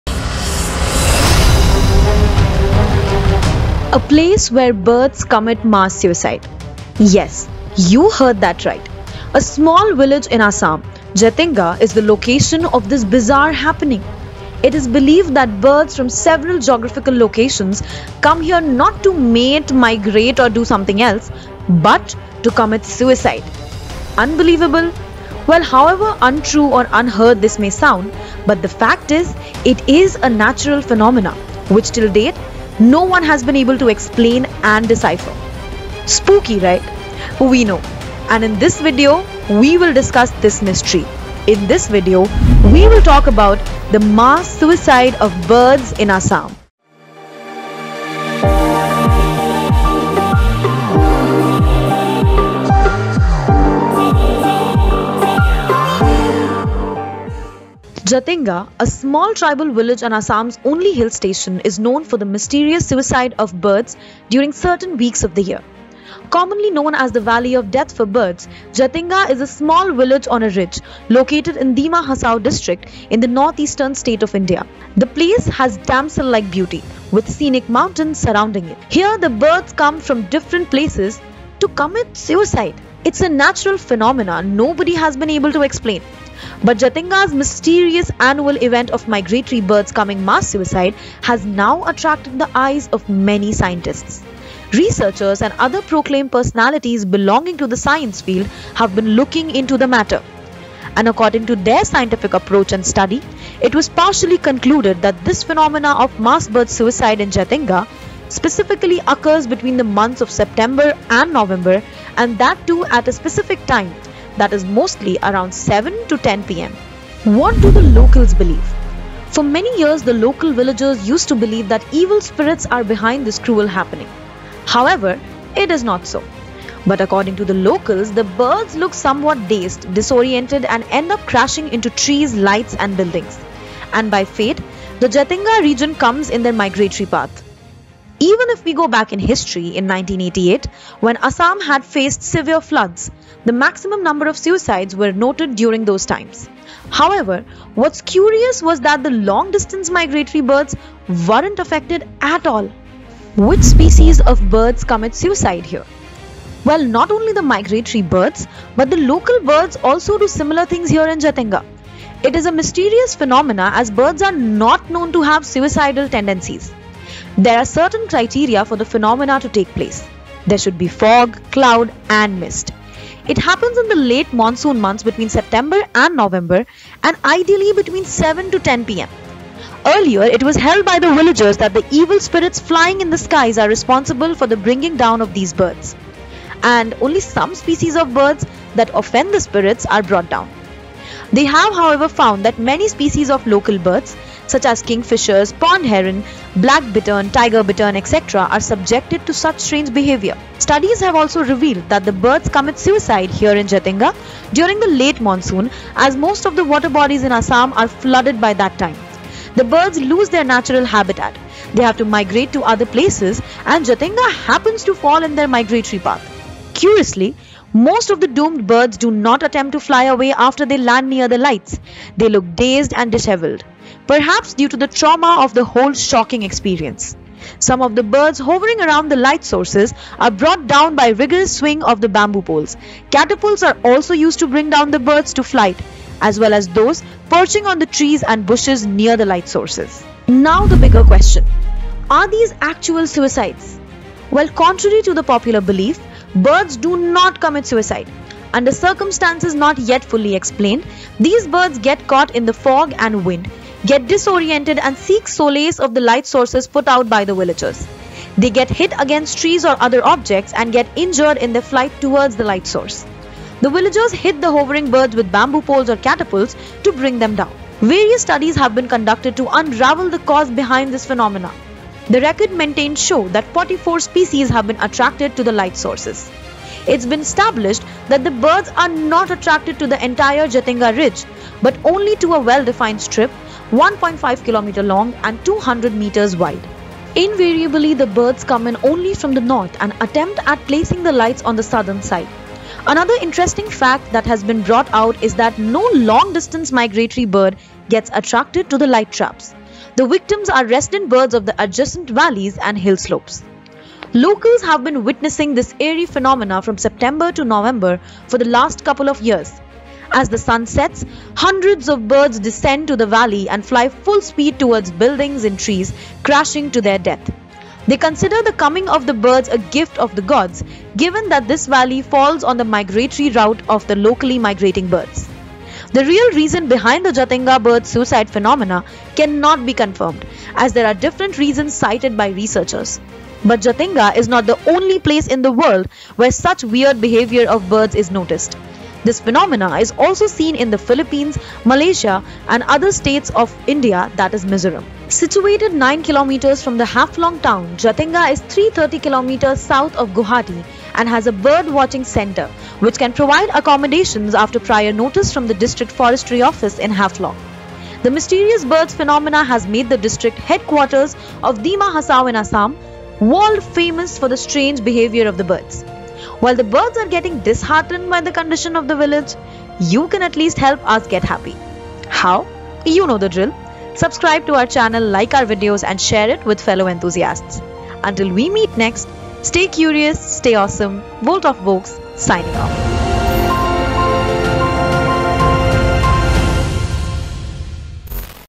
The Mass Suicide Of Birds In Assam ! | Vault Of Vox
Jatinga, a small tribal village and Assam’s only hill station, is known for the mysterious suicide of birds during certain weeks of the year. Commonly known as valley of death for birds, Jatinga is a small village on a ridge, is located in Dima Hasao district (DHD), in the north-eastern state of India, Assam. The place has damsel like beauty with scenic mountains surrounding it. Here the Birds come from different places to commit suicide. It's a natural phenomenon nobody has been able to explain. But Jatinga's mysterious annual event of migratory birds committing mass suicide has now attracted the eyes of many scientists.
Researchers and other proclaimed personalities belonging to the science field have been looking into the matter. And according to their scientific approach and study, it was partially concluded that this phenomenon of mass bird suicides in Jatinga, Assam specifically occurs between the months of September and November and that too at a specific timing, i.e., mostly around 7 pm to 10 pm.
What do the locals believe?
For many years, the local villagers used to believe that evil spirits are behind this cruel happening. However, it is not so. But according to the locals, the birds look somewhat dazed, disoriented and end up crashing into trees, lights and buildings.
And by fate, the Jatinga region comes in their migratory path. Even if we go back in history, in 1988, when Assam had faced severe floods, the maximum number of suicides were noted during those times. However, what's curious was that the long-distance migratory birds weren’t affected by this at all.

Which species of birds commit suicide here?

Not only the Migratory birds, but the local birds also do similar things here in Jatinga.
It is an mysterious phenomenon as birds are not known to have suicidal tendencies. There are certain criteria for this phenomenon to take place; there should be fog, cloud and mist. It happens in the late monsoon months between September and November and ideally between 7 p.m. to 10 p.m. Earlier it was held by the villagers that the evil spirits flying in the skies are responsible for bringing these birds down. And only some species of birds that offend the spirits are brought down.

0:00 - Intro
 Jatinga, a small tribal village and Assam’s only hill station 01:00
  But Jatinga's mysterious annual event of migratory birds committing mass suicide now
 attracted the eyes of many scientists. 01:28
  What do locals believe in Jatinga? 01:58
  the birds look somewhat dazed, and disoriented and end up crashing  02:09
  when Assam faced severe floods, the maximum number of suicides was noted  02:23
  Which species of birds commit suicide here? 02:36
  Studies have also revealed that the birds commit suicide in Jatinga during the late monsoon   03:26
 Curiously, most of the doomed birds do not attempt to fly away 03:40
  Some of the birds hovering around the light sources are brought down 03:48
  Catapults are also used to bring down the birds in flight  03:53
  So are these actual suicides? 04:01
 The villagers hit the hovering birds with bamboo poles  04:24
  The record maintained show that 44 species have been attracted to the light sources 04:34
   The victims are resident birds of the adjacent valleys and hill slopes 05:01
   The real reasons behind the Jatinga bird's suicide phenomenon cannot be confirmed 05:26
   But Jatinga is not the only place in the world where such weird behavior of birds is noticed.  05:35
   While the birds are getting disheartened by the conditions of the village 06:08